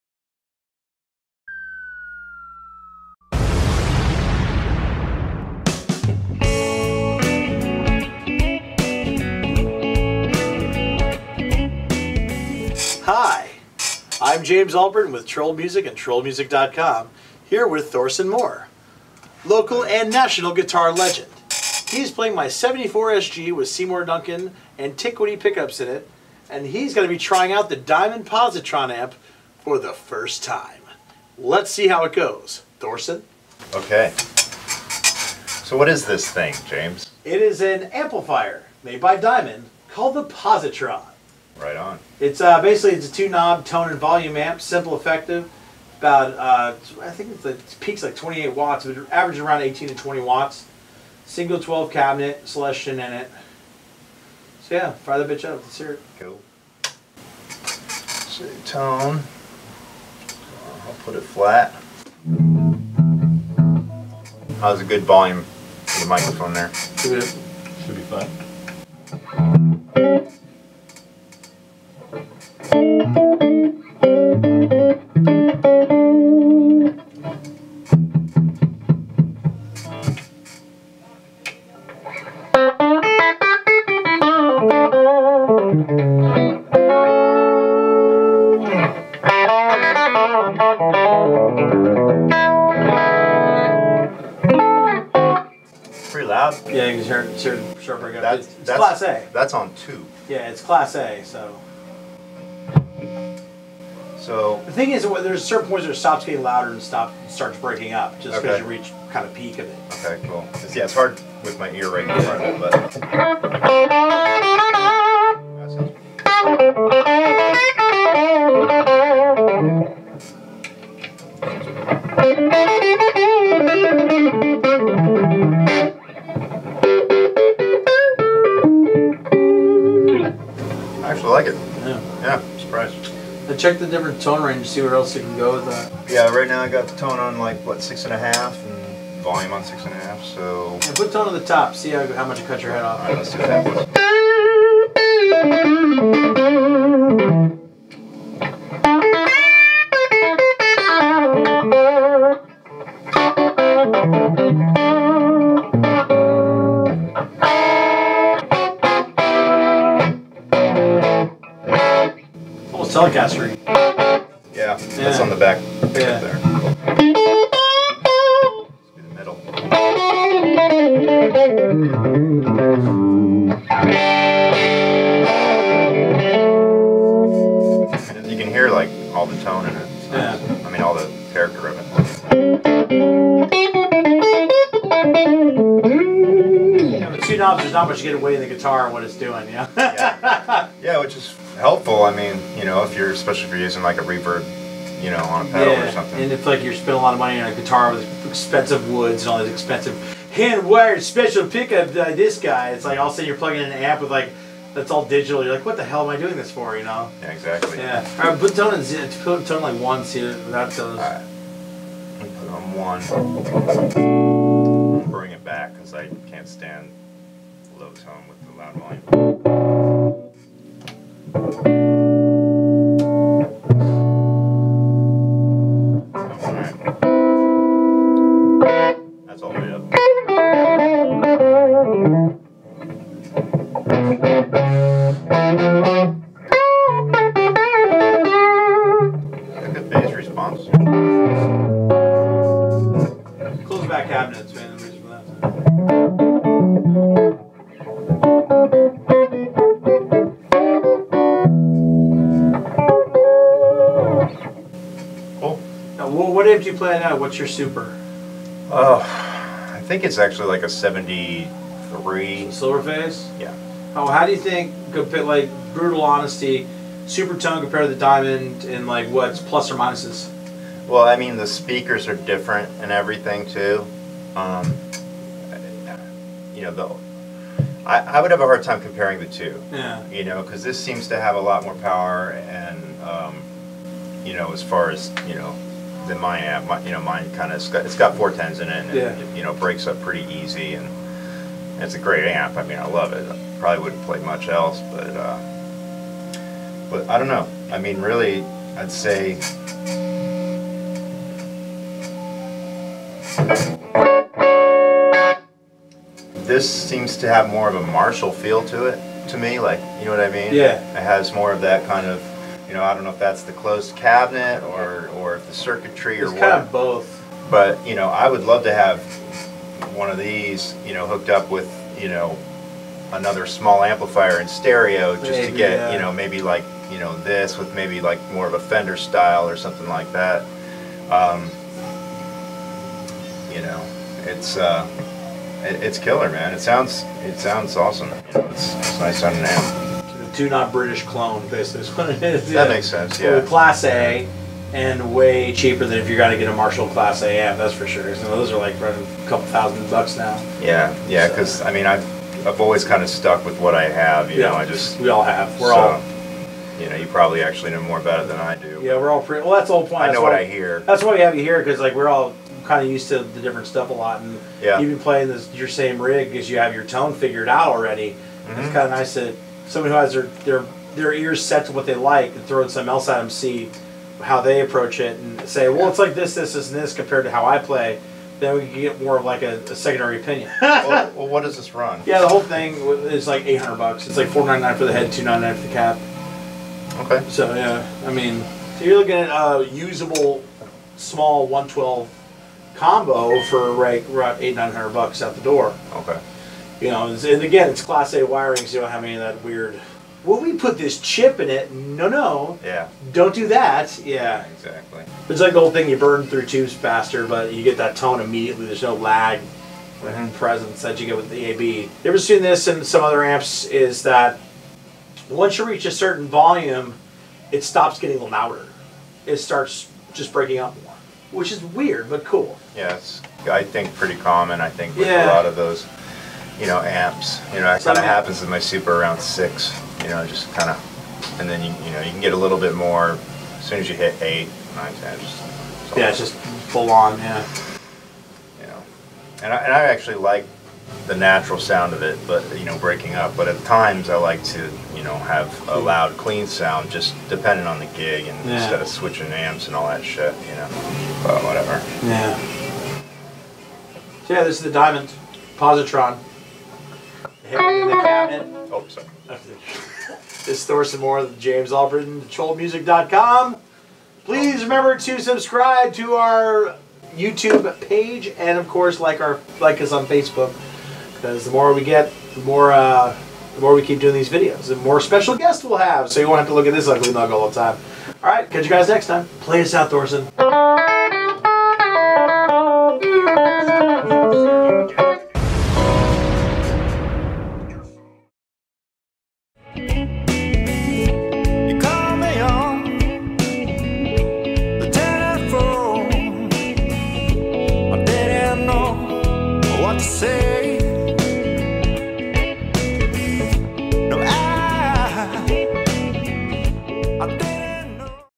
Diamond Positron Amp
James Albritton discusses the Diamond Amplification Positron Amp with Sarasota, FL based guitarist and local music hero Thorson Moore.